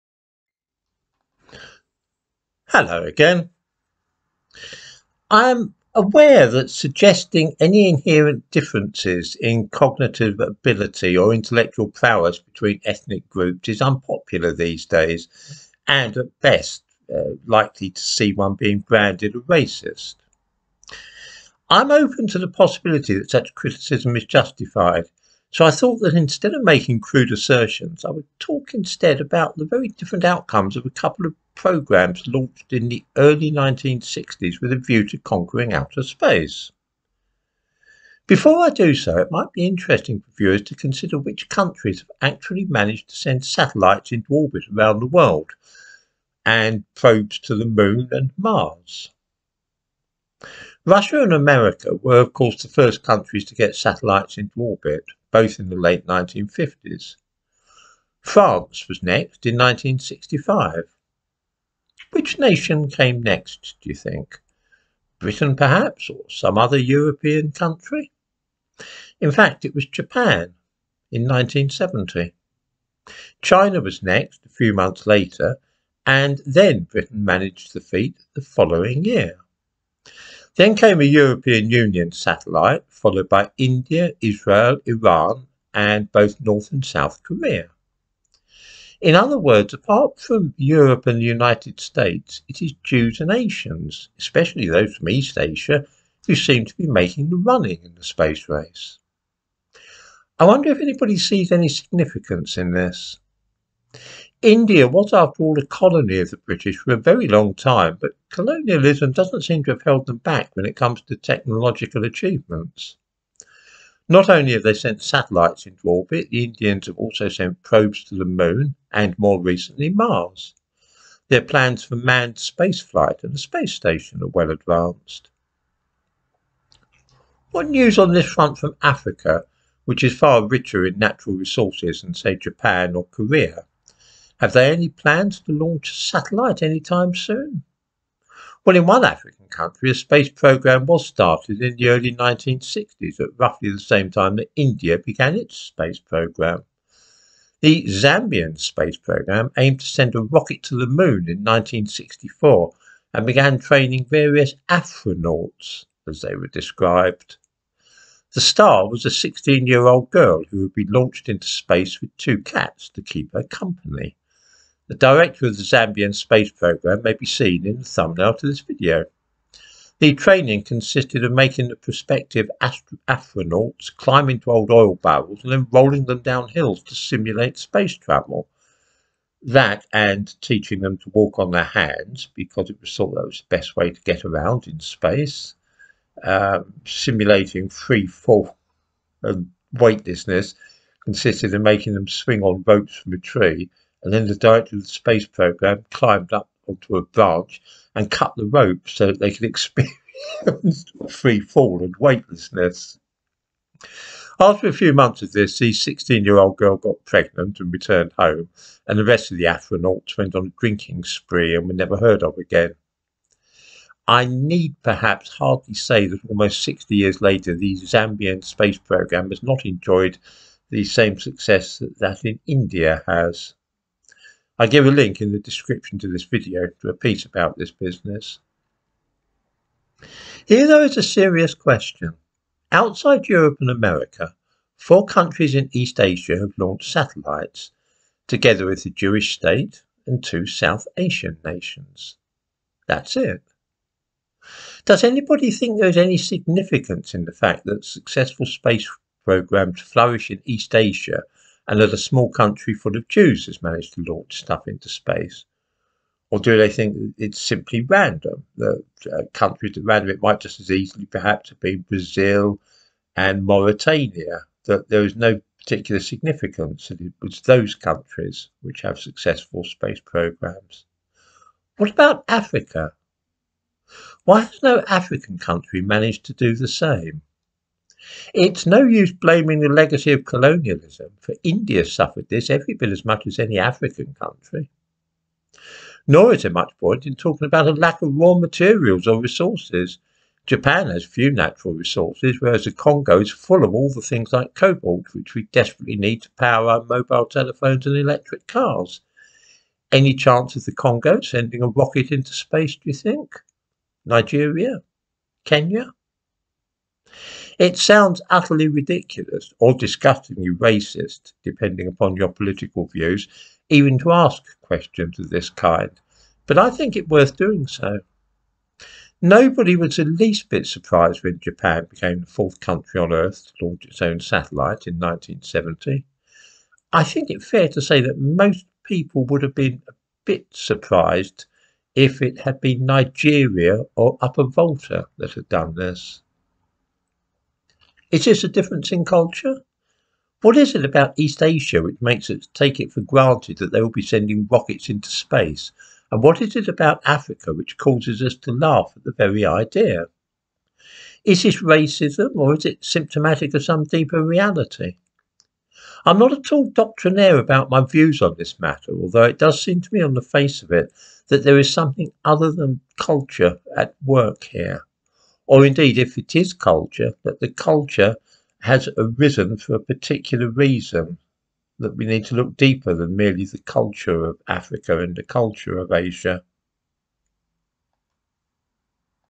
The difference between space programmes in Asia and Africa
Attempts to launch satellites and probes to the moon have been made by various countries. The differences between the sophistication of these programmes in India and Japan, as against those in African nations is stark.